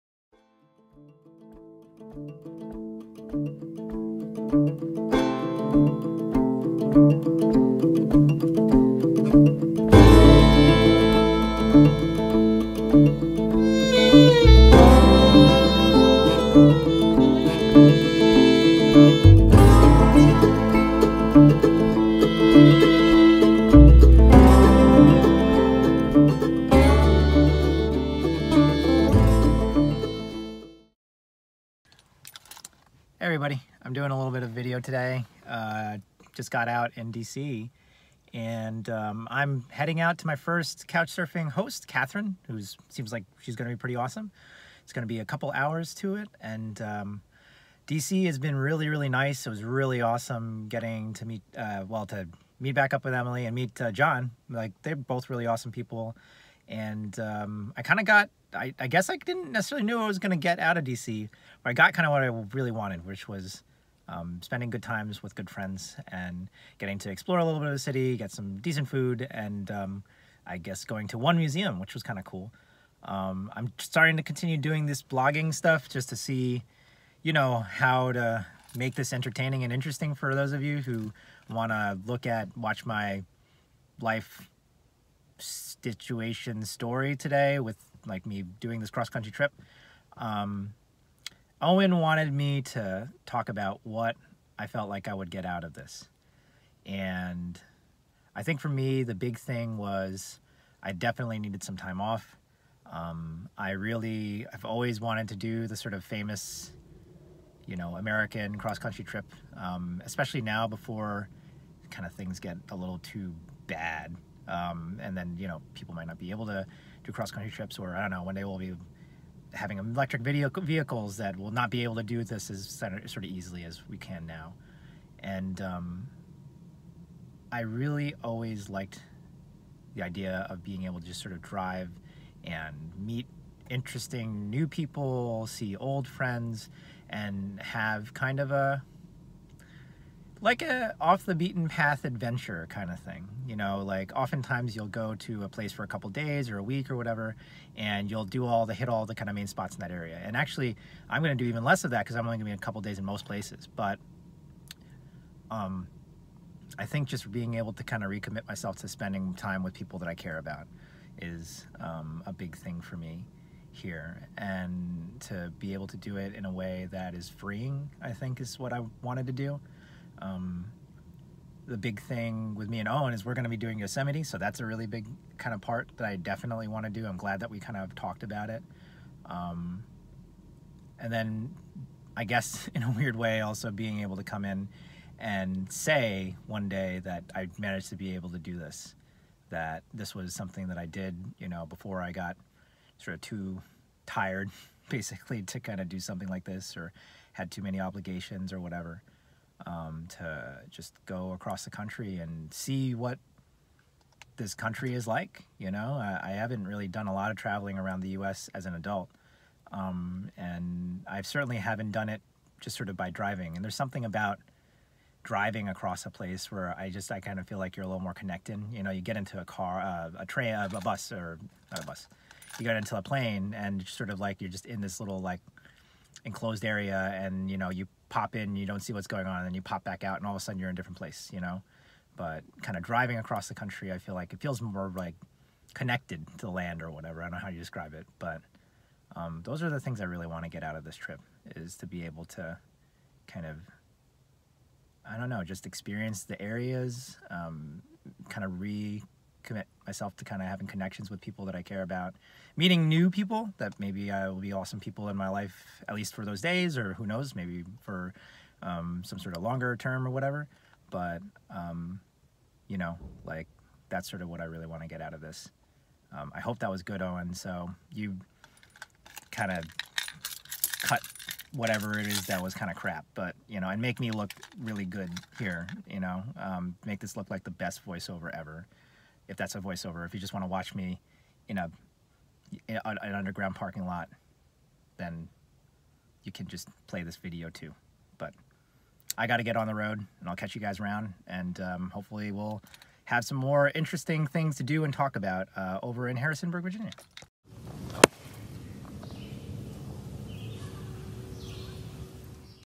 Jon Travels - Friends, Food, Fun - the simple pleasures of life - DC - Day 2.2
A part of the Lazy Bushcrafters series - follow Jon's adventure as he makes his way across America and back!